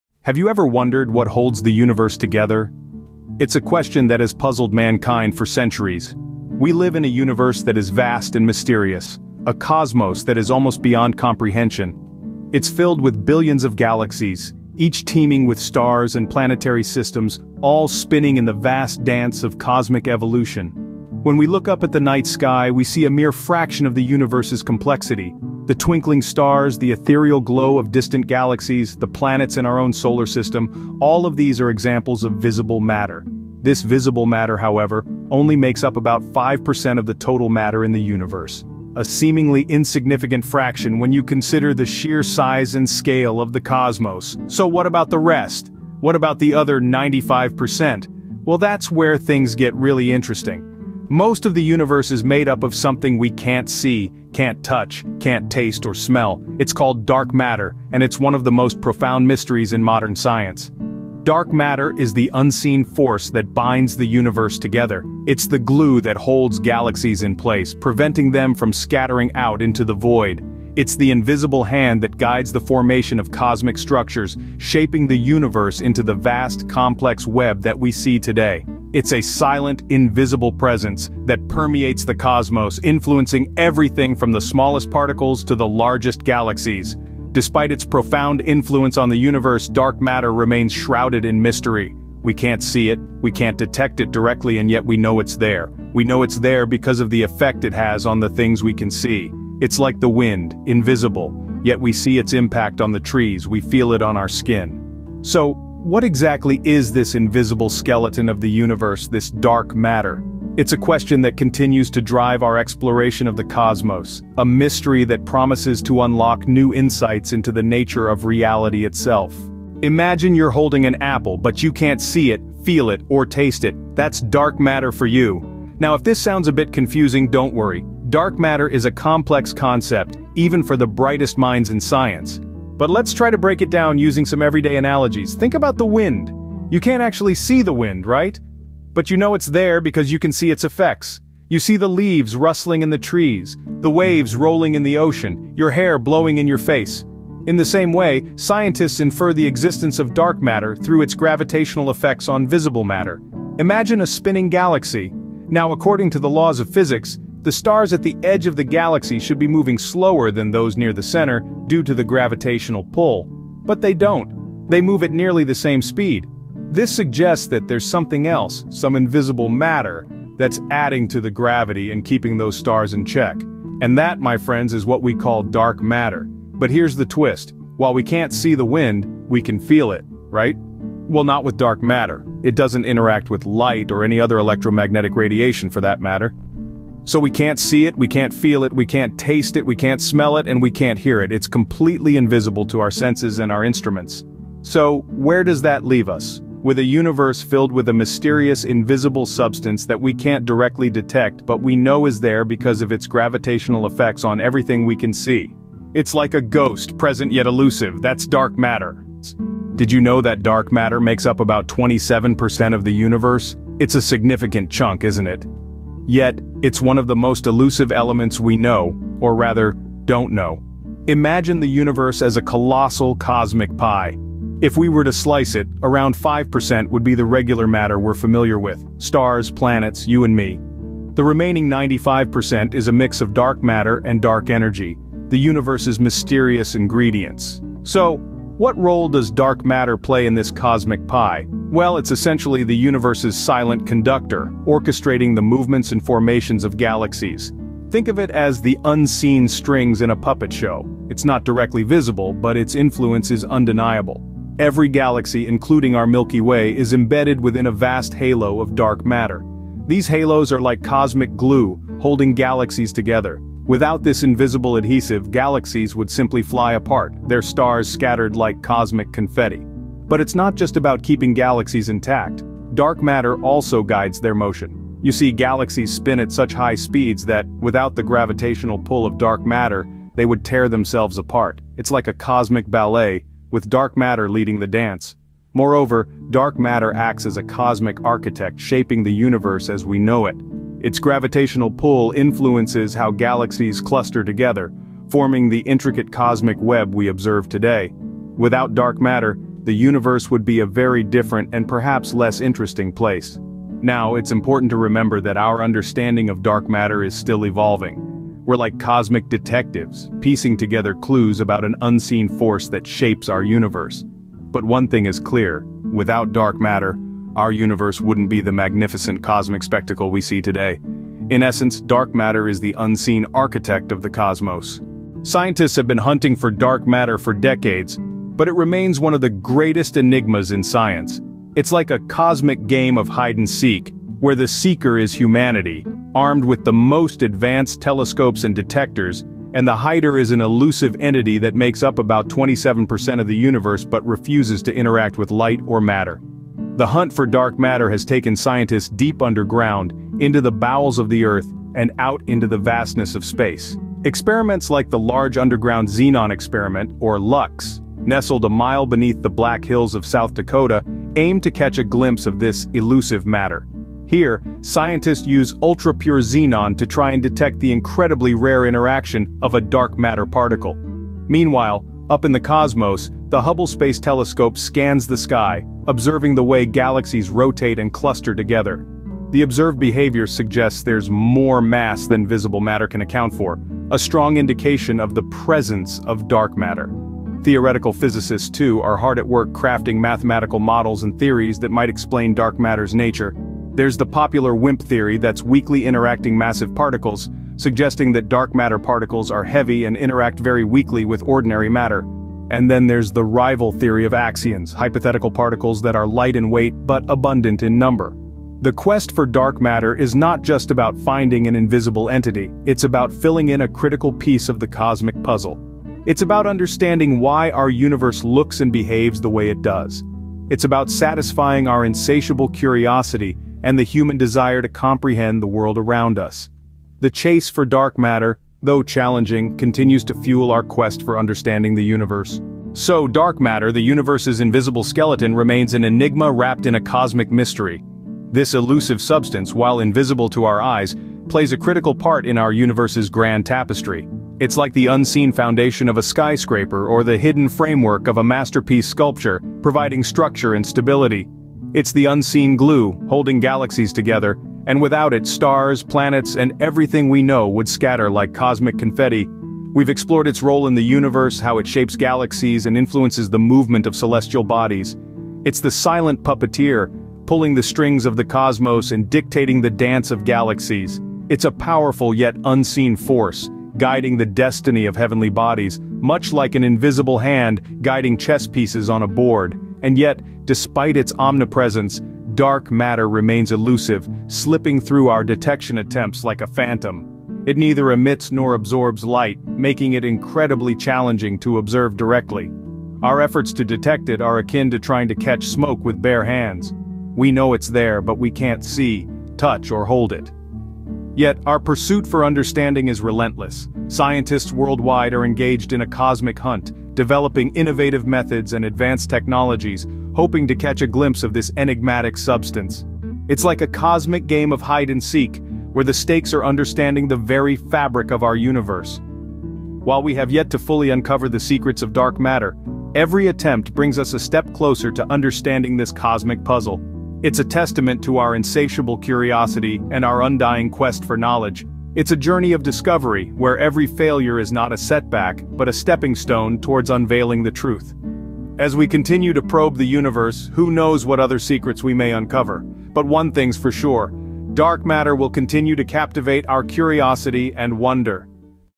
The Mystery of Dark Matter Universe's Invisible Skeleton
Delve into the fascinating world of dark matter, the invisible skeleton that holds the universe together. Explore the mysteries of this elusive substance and its impact on our understanding of the universe in this captivating video.